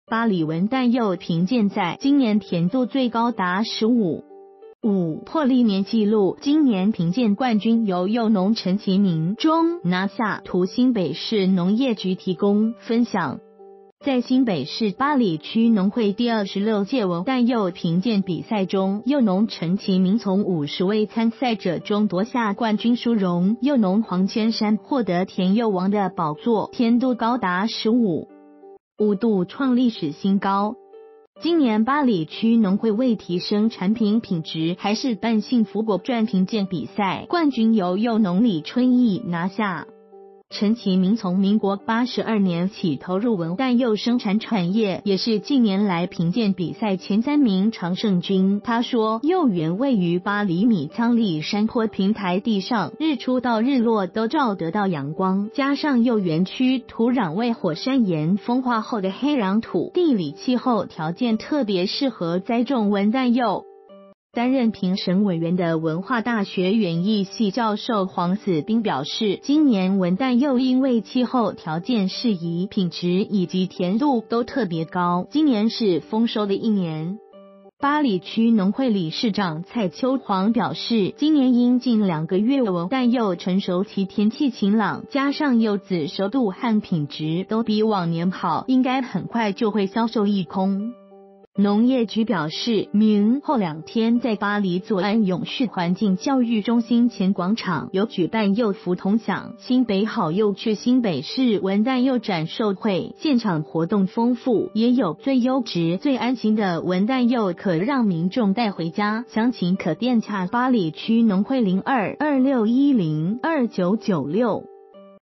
八里文旦柚 甜度15.5創新高
八里文旦柚評鑑賽，今年甜度最高達15.5，破歷年紀錄，今年評鑑冠軍由柚農陳旗明（中）拿下。 圖／新北市農業局提供 分享

發表評論評論如下。